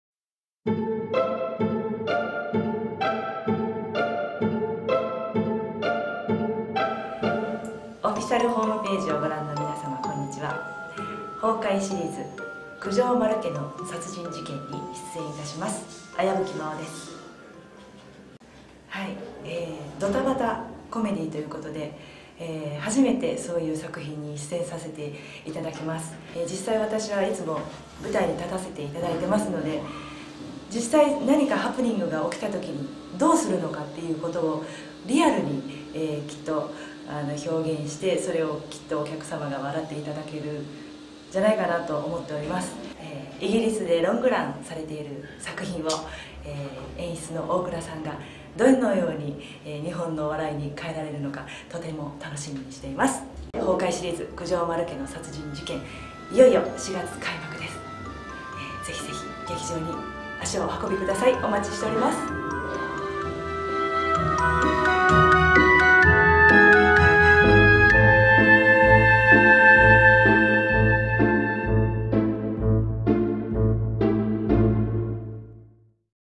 ～崩壊シリーズ～九条丸家の殺人事件－彩吹真央
賞味100分の崩壊喜劇！2015年英国オリヴィエ賞最優秀コメディ賞受賞！イギリスでロングラン上演中のコメディが早くも日本初上陸！2016年4月、東京ほか全国にて上演。